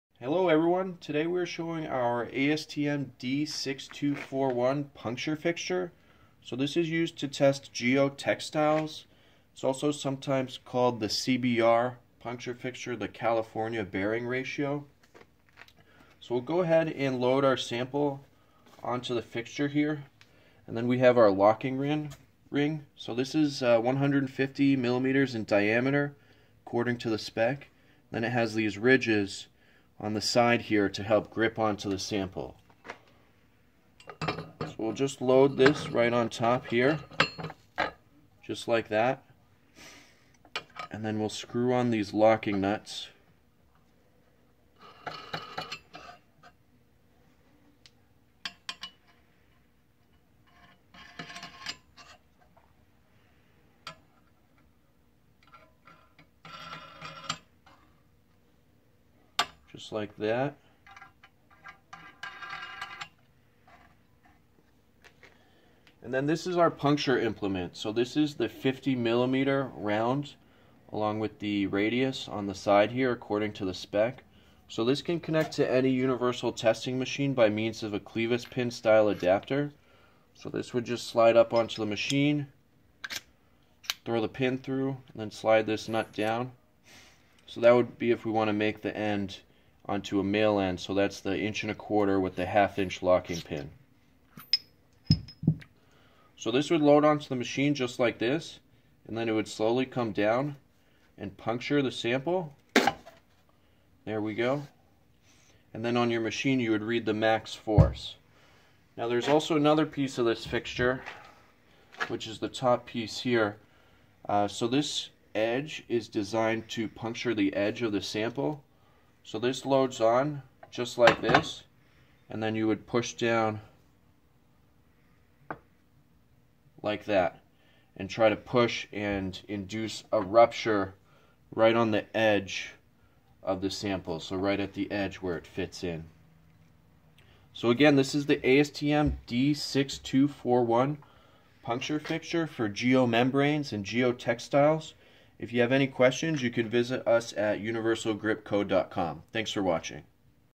ASTM D6241 Puncture Fixture for Geotextiles
The ASTM D6241 Puncture Fixture is used for testing geotextiles on a universal testing machine. This test is also commonly referred to as the California Bearing Ratio or CBR test. The puncture plunger is 50mm in diameter with the chamfered edges according to the specification.